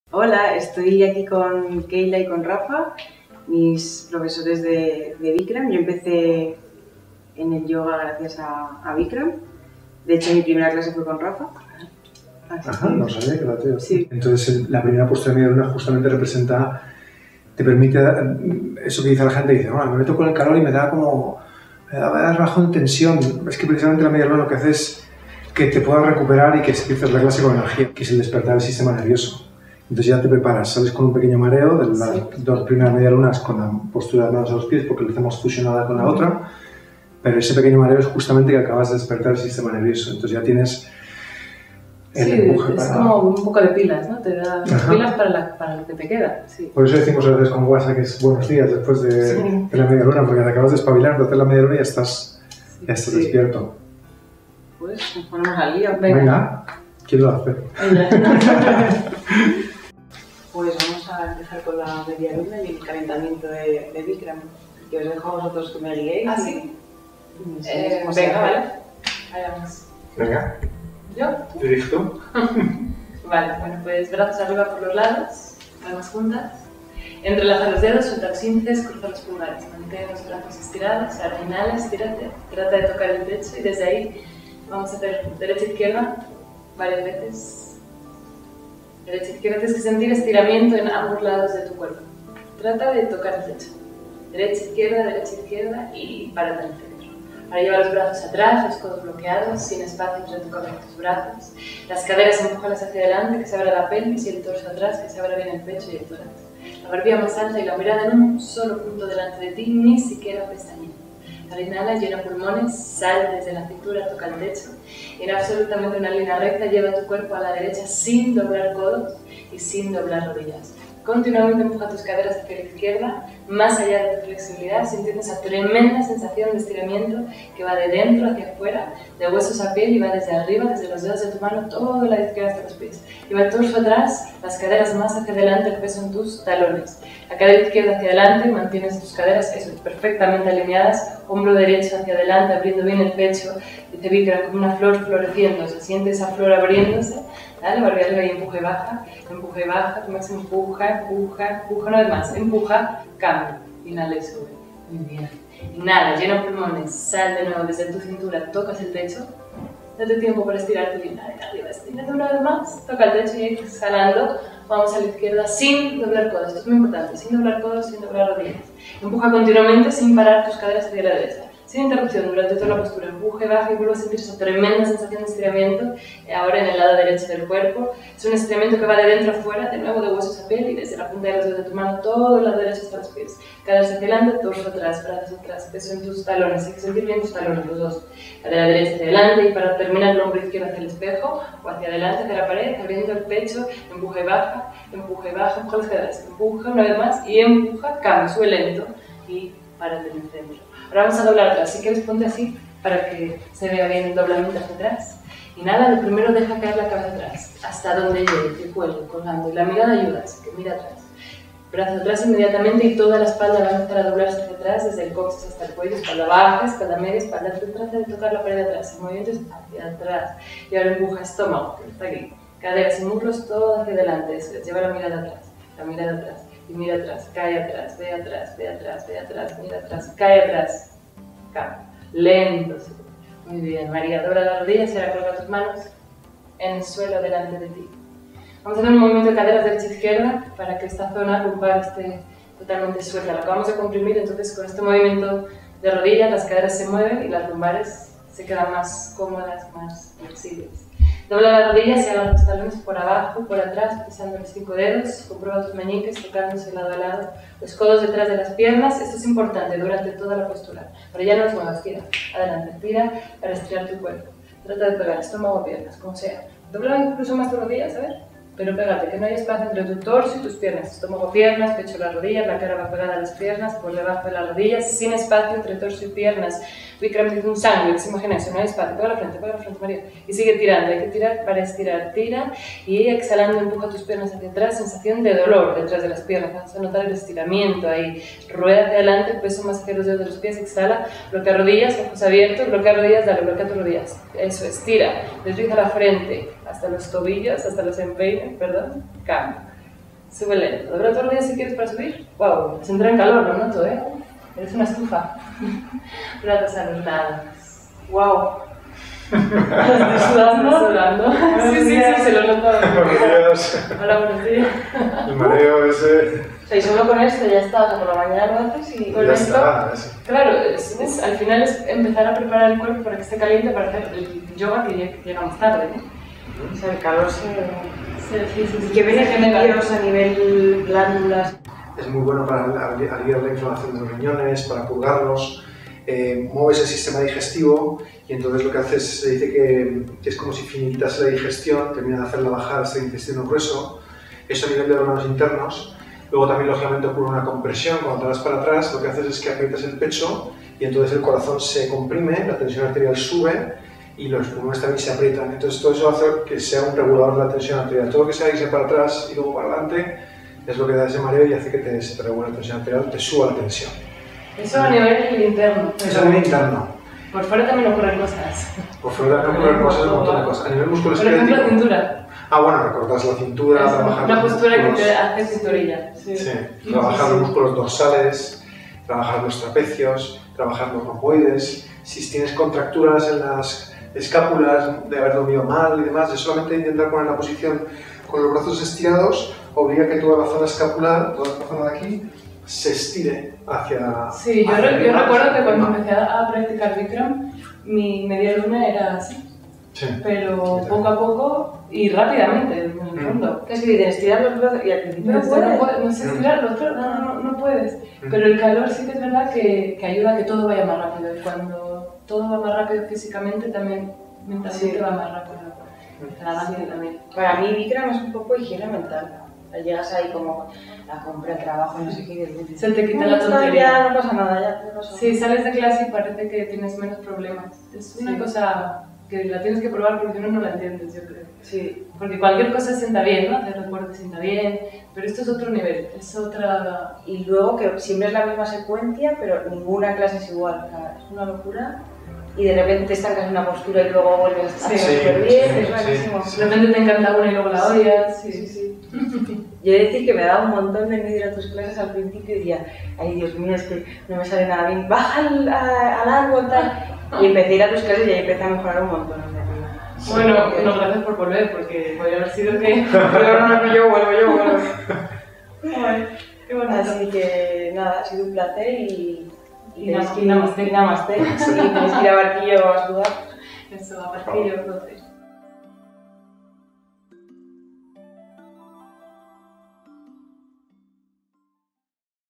Calentamiento de media luna para bikram yoga
Hoy puedes ver a Maria Rosenfeldt hacer un calentamiento de Bikram Yoga con Keila Velon y Rafael Veiga de Barquillo Hot Yoga. Con esta experiencia de media luna busca preparar tu cuerpo para las exigencias del bikram yoga en tu práctica.

MOMENTOS:
00:00 María, Keila y Rafael
01:00 Rutina de Yoga